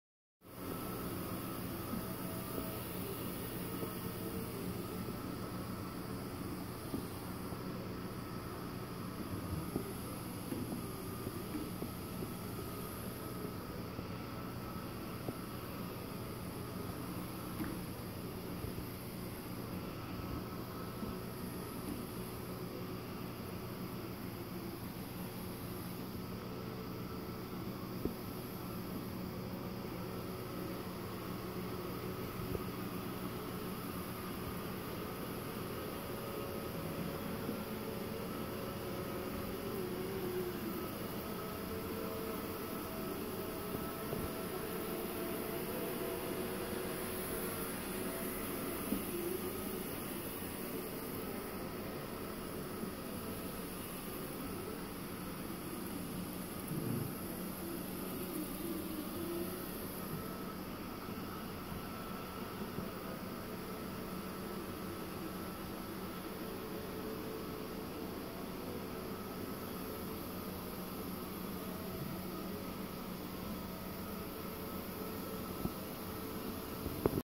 Manually Controlling Ecovacs Deebot N79S with the Remote Control - Spot Cleaning Rug
I was testing out the manual control feature of the handheld remote controller for the Ecovacs Deebot N79S robot vacuum cleaner. You can also use the app to do the same thing. I wanted to see how well the robot could pick up dog hair on the rug.